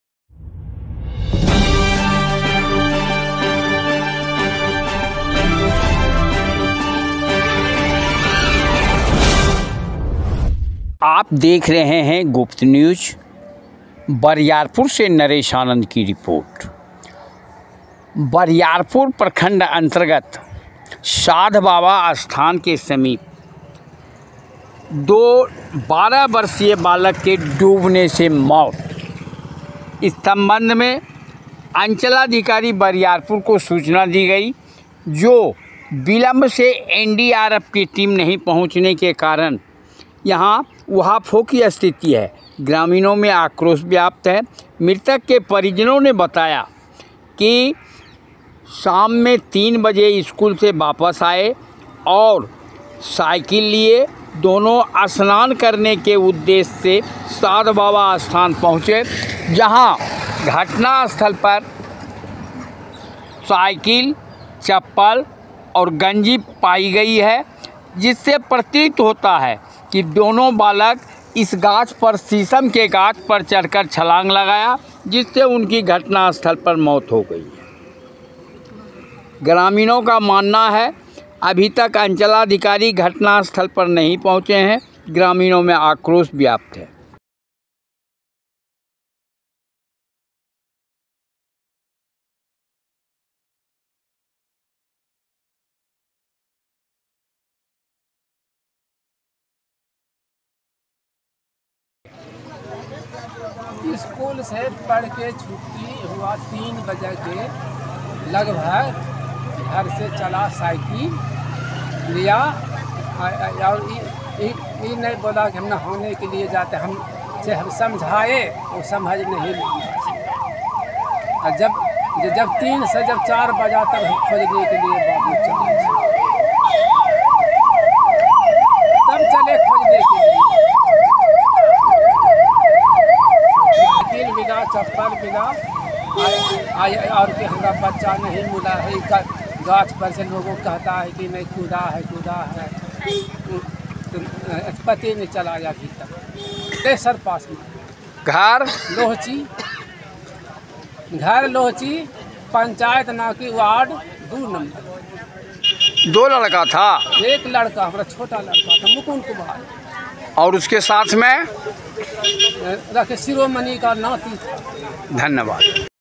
बरियारपुर प्रखंड के अंन्तर्गत दो बारह वर्षीय बालक के डुबने से म...त परिजनौ मे मची कौहराम।बरियारपुर
बरियारपुर प्रखंड के अंन्तर्गत दो बारह वर्षीय बालक के डुबने से म...त परिजनौ मे मची कौहराम।
बरियारपुर